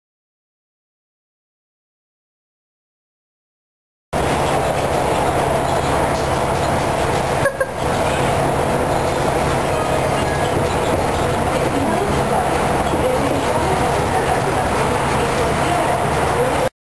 ::-:: GATO ANIMADOR - LOCURAS 3 ::-::
Solito en el metropolitano, 10:30pm. Por suerte pude entrar al famoso bus que cierra a las 9:30pm . Créditos: Camara - Luis Condori. Lunes 22-11-10.